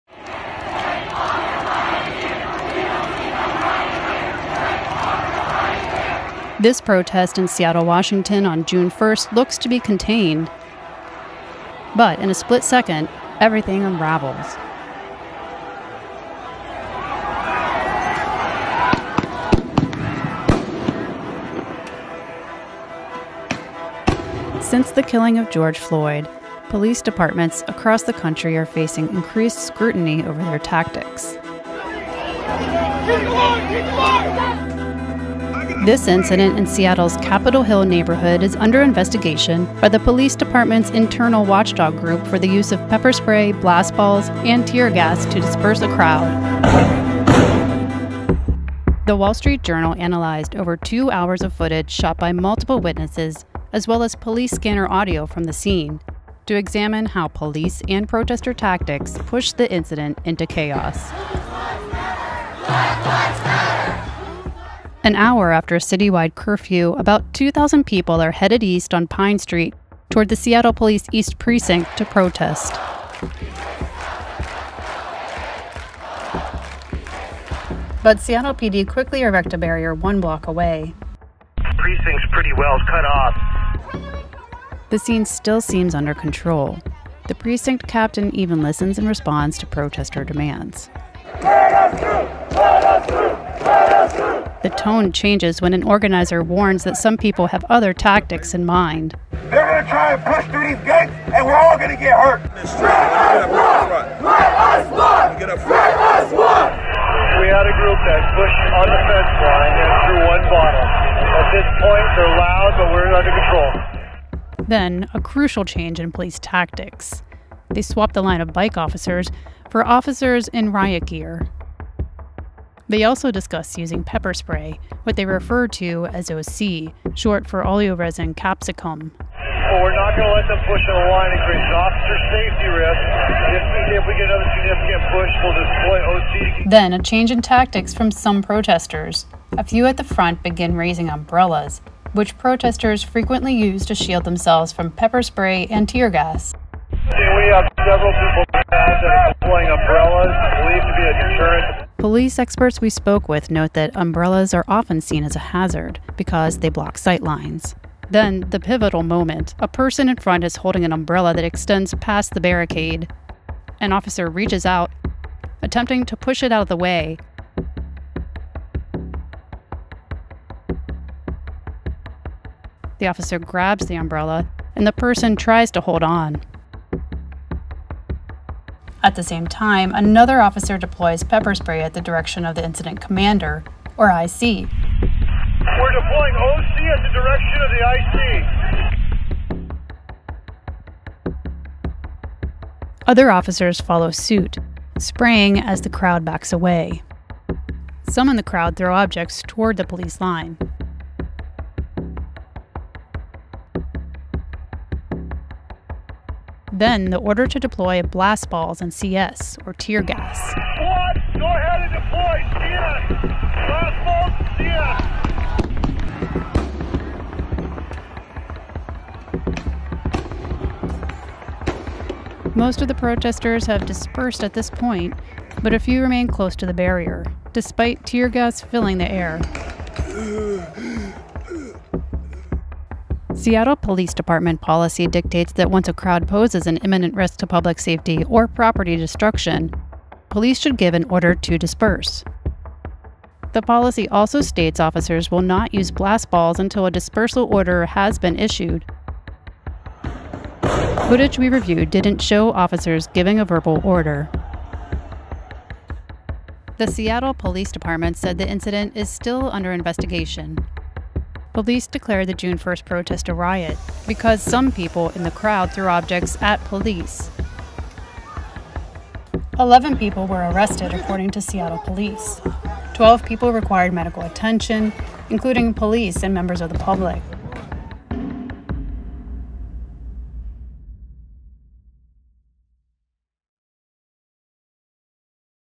Video Investigation: How a Seattle Protest Ended in Chaos | WSJ
On June 1, a largely peaceful demonstration in Seattle turned violent, with police using pepper spray and tear gas against the protesters. Using video and police scanner audio, WSJ dissects how the event unfolded and why it's now under investigation.

Photo: Lindsey Wasson/Reuters

WSJ video investigations use visual evidence to reveal the truth behind the most important stories of the day.